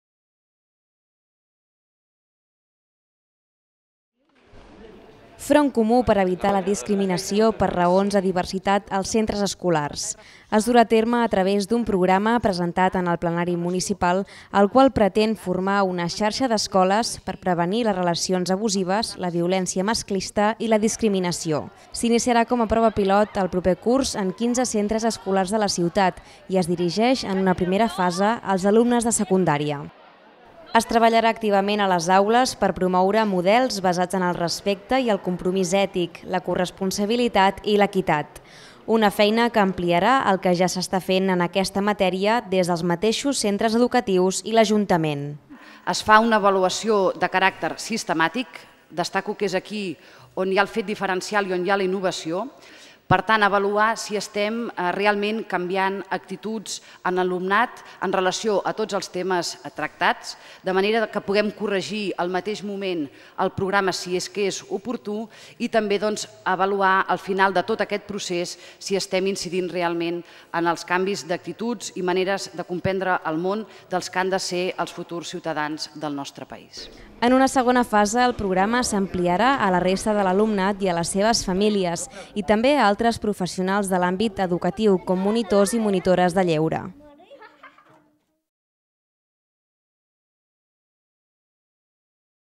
Front comú per evitar la discriminació als centres escolars
La prova pilot del programa, presentat al Plenari Municpal, s’iniciarà el curs vinent a 15 centres de la ciutat. Es dirigeix en una primera fase a alumnes i professorat de secundària. Es treballarà activament a les aules per promoure el respecte i el compromís ètic. Més informació a